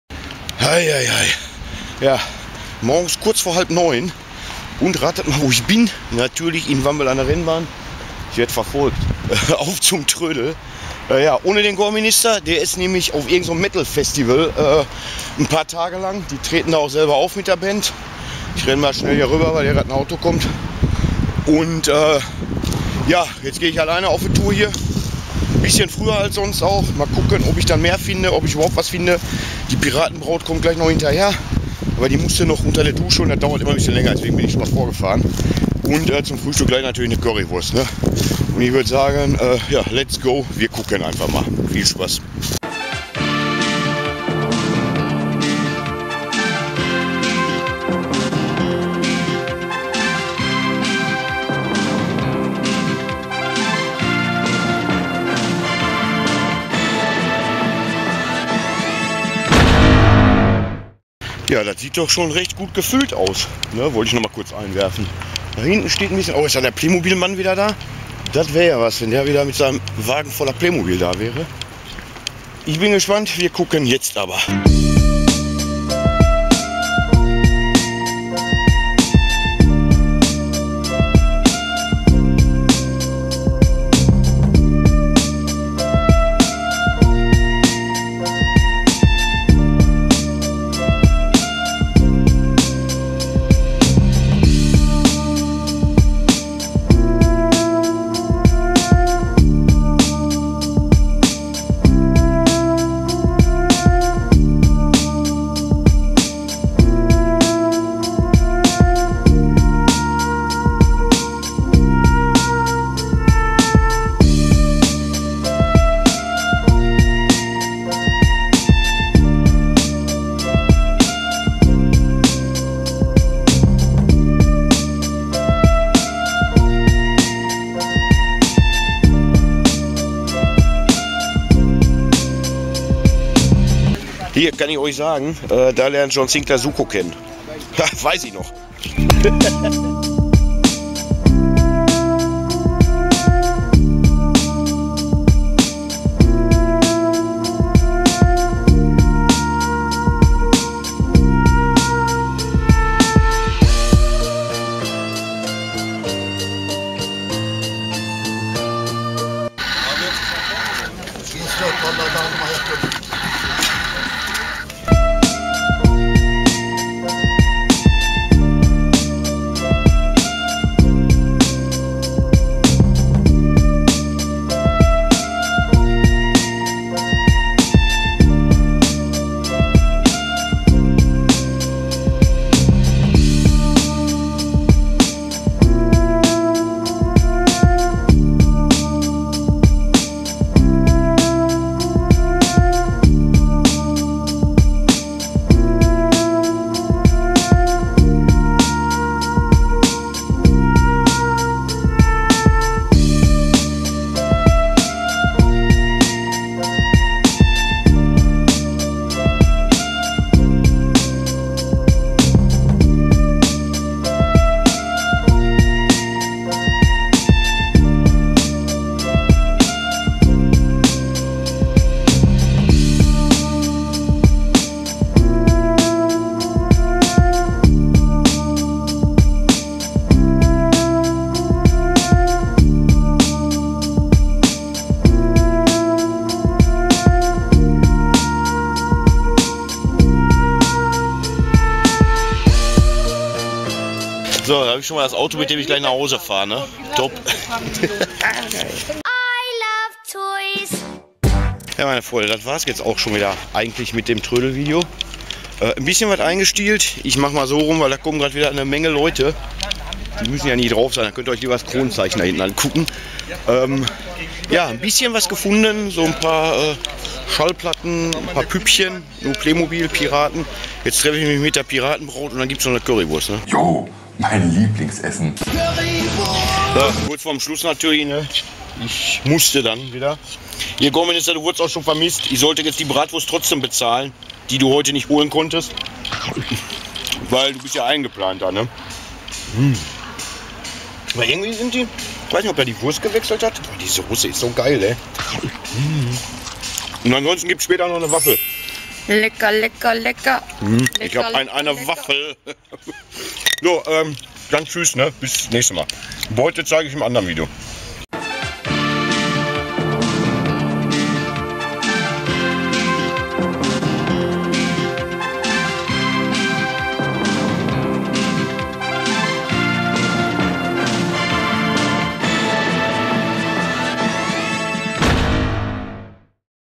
AUFFE TRÖDELTOUR (11.AUG.2022 - PFERDERENNBAHN DO-WAMBEL)
Und wieder eine kleine Trödeltour auf'm DSD sein Stammtrödelmarkt in Dortmund Wambel anne Pferderennbahn.Eine kleine Auswahl von für mich interessanten Dingen und natürlich auch wieder Currywurstverzehrung.. ...